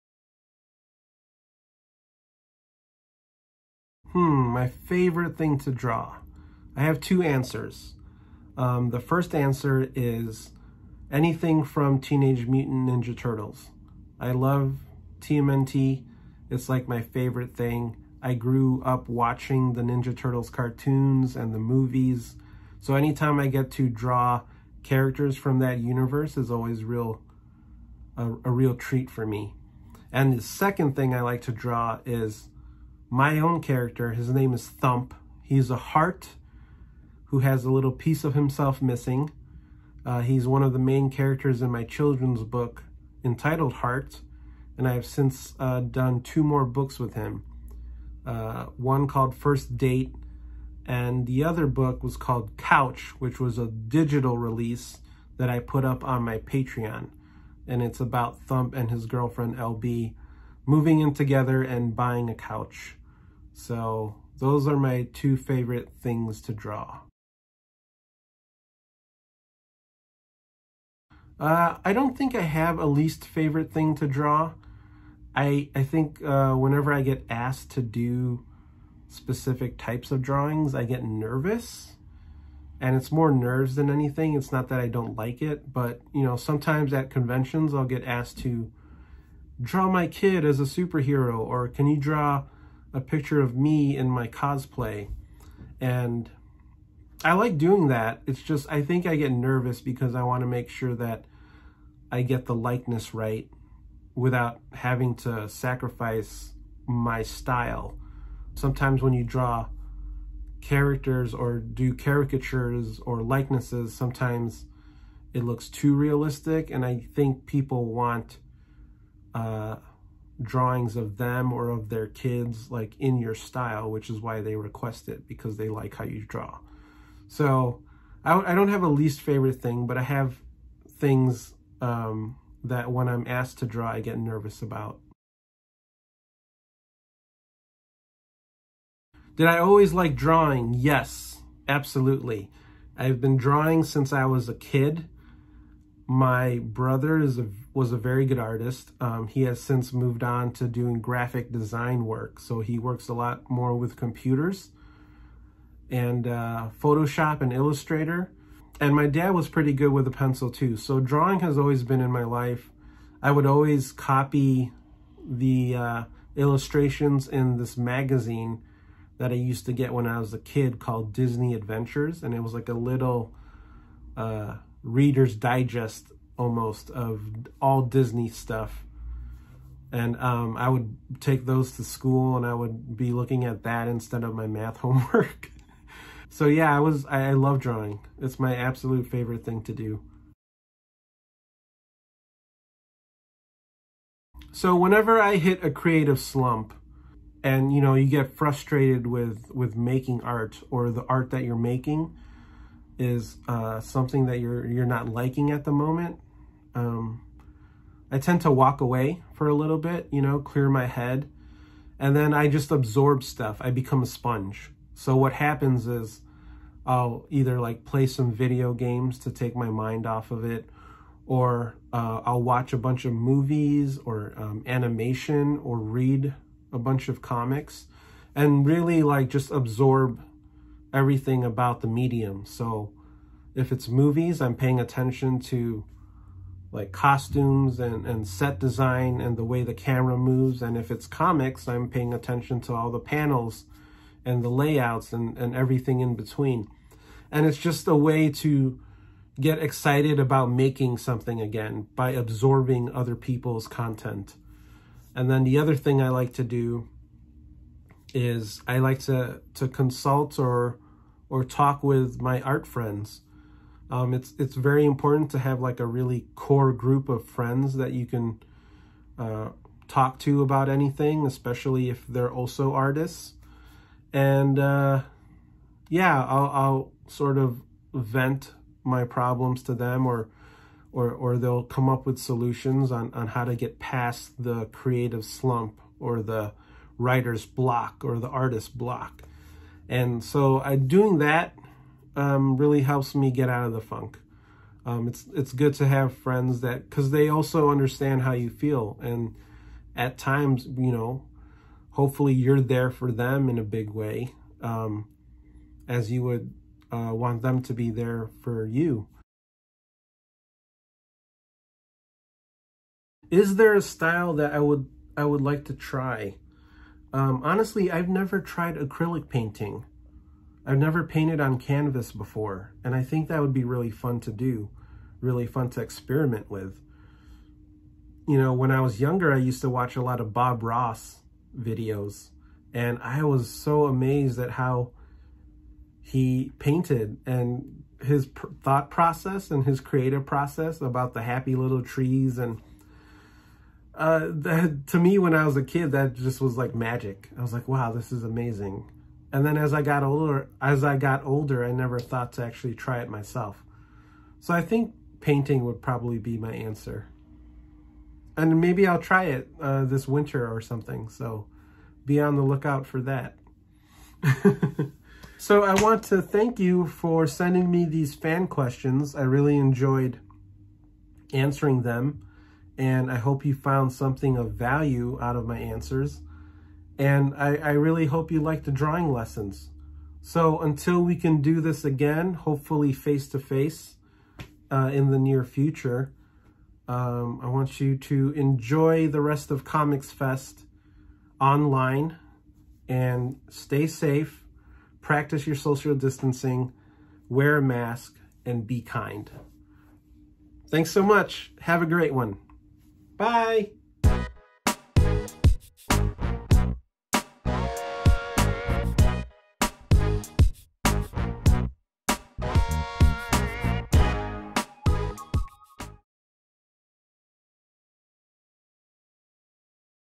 ComicFest 2020: Answering Fan Questions!
I answer some of your questions. I give you a little history of my love for drawing and some helpful tips on how to come out of the creative slump. Thanks!